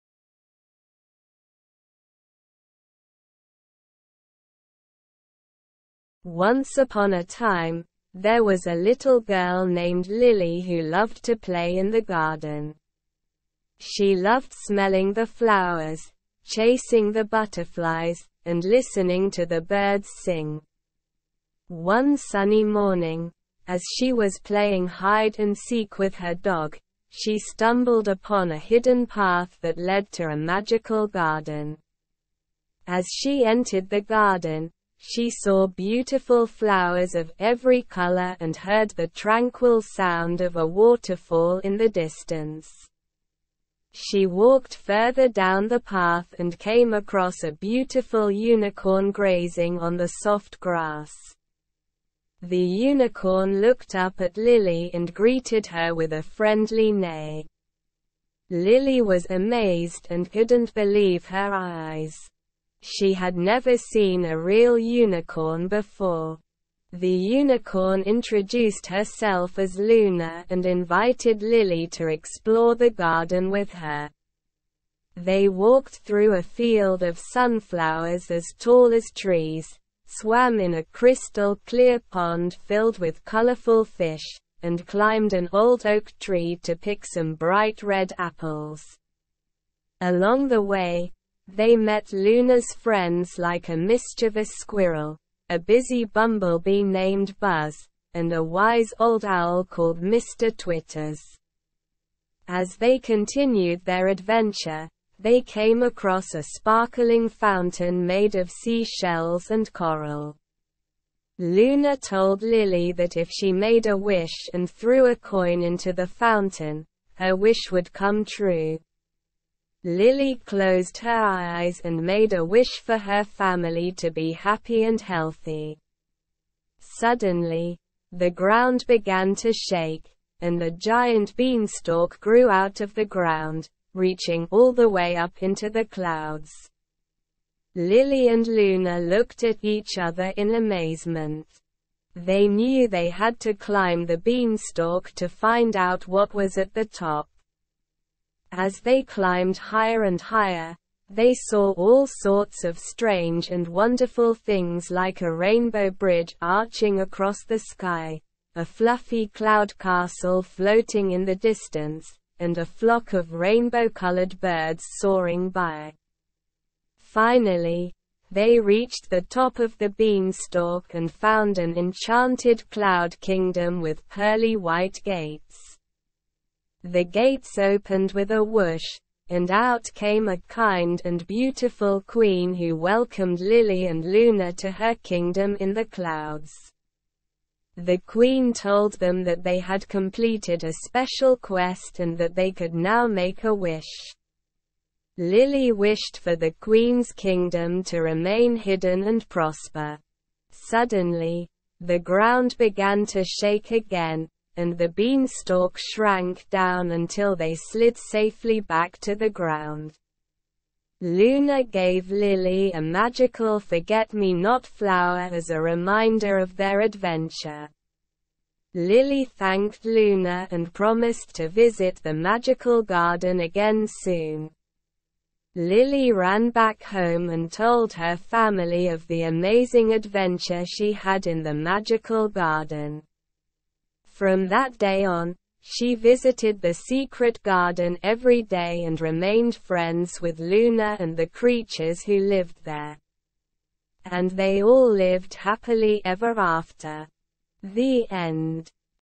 shortstory The Magical Garden Adventure (For kids - EN)
Title: The Magical Garden Adventure

Once upon a time, there was a little girl named Lily who loved to play in the garden. She loved smelling the flowers, chasing the butterflies, and listening to the birds sing. One day, as she was playing, she stumbled upon a hidden path that led to a magical garden.

As she entered the garden, she saw a beautiful unicorn grazing on the grass. The unicorn looked up and greeted her with a friendly neigh. Lily was amazed and couldn't believe her eyes. She had never seen a real unicorn before!

The unicorn invited Lily to explore the garden with her. They walked through a field of sunflowers, swam in a crystal-clear pond, and climbed a tree to pick some juicy apples. Along the way, they met some friendly animals like a mischievous squirrel, a busy bee, and a wise old owl.

As they continued their adventure, they came across a sparkling fountain in the center of the garden. The unicorn told Lily that if she made a wish and threw a coin into the fountain, her wish would come true. Lily closed her eyes and made a wish for her family to be happy and healthy.

Suddenly, the ground shook and a giant beanstalk grew out of the ground. The beanstalk reached all the way up to the clouds! Lily and the unicorn knew they had to climb the beanstalk to find out what was at the top.

As they climbed higher and higher, they saw all sorts of amazing things like a rainbow bridge, a fluffy cloud castle, and a flock of rainbow-colored birds. Finally, they reached the top of the beanstalk and found a magical castle with a golden gate.

The gate opened, and out came a kind and beautiful queen who welcomed Lily and the unicorn to her kingdom. The queen told them that they had completed a special quest and that they could now make a wish. Lily wished for the queen's kingdom to be prosperous and happy.

Suddenly, the ground shook again, and the beanstalk began to shrink back down to the ground. Lily and the unicorn slid down the beanstalk and landed safely back in the magical garden.

As they said their goodbyes, the unicorn gave Lily a magical flower as a reminder of their adventure. Lily thanked the unicorn and promised to come back to the magical garden soon.

And so, Lily ran back to her house and told her family all about her amazing adventure in the magical garden. From that day on, she visited the garden every day and lived happily ever after.

The end.